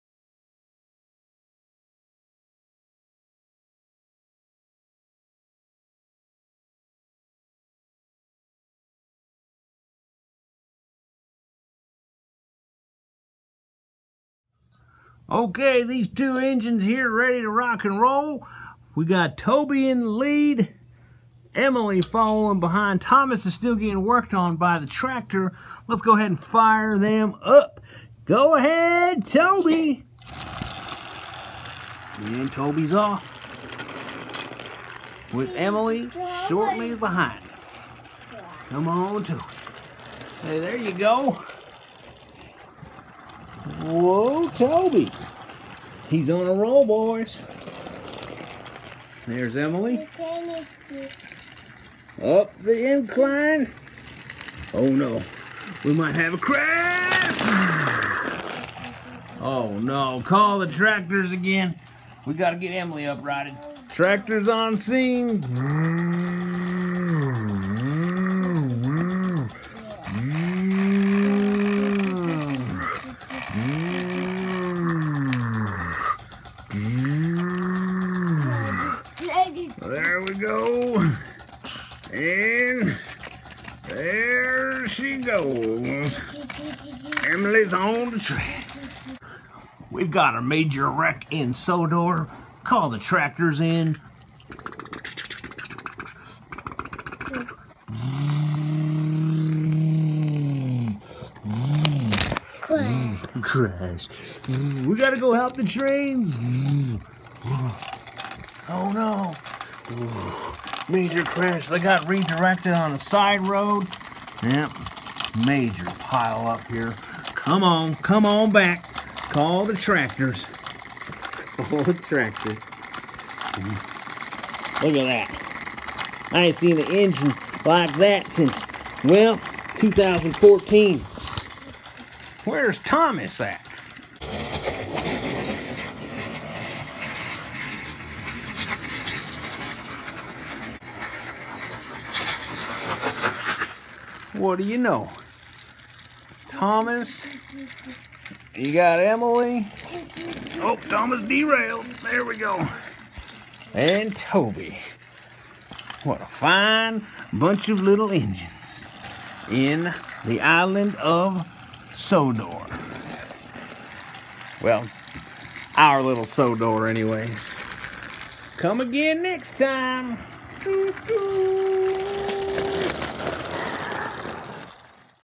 Thomas and Emily get help from Tractors.
Jer Bear and his dad play with Thomas the Train and use Tractors to help after things get a little messy.